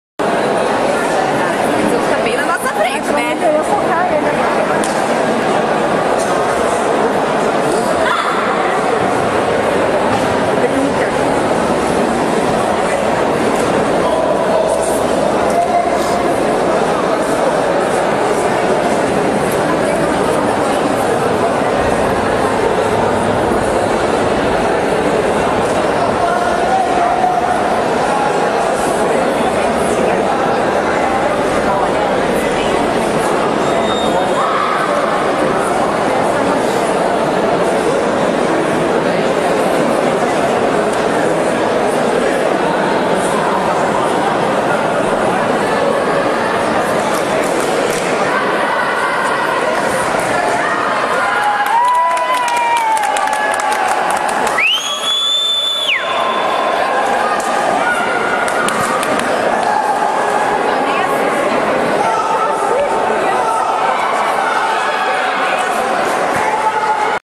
Rhayza Taekwondo
Vídeo da minha primeira competição de Poomse. Faixa Vermelha Ponta preta.